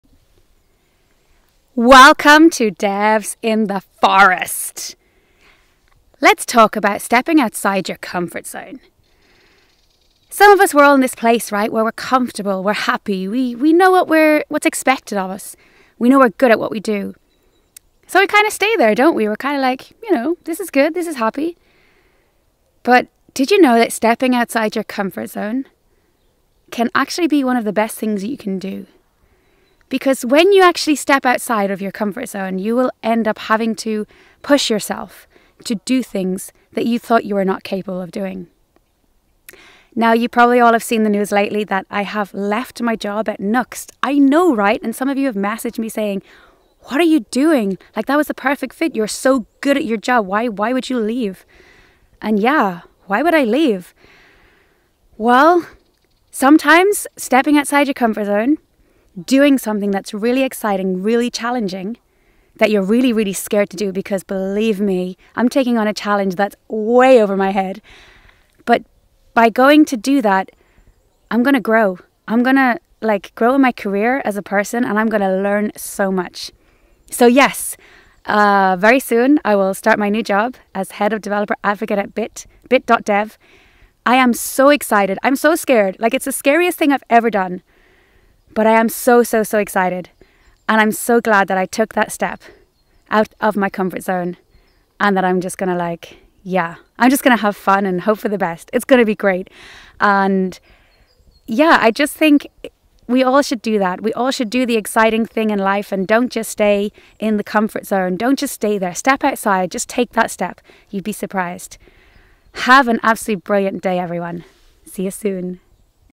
Devs in the Forest: Be Uncomfortable
Step outside your comfort zone and start to feel uncomfortable as that is when you will start to grow and progress. It's not easy of course but when you do it great things will happen.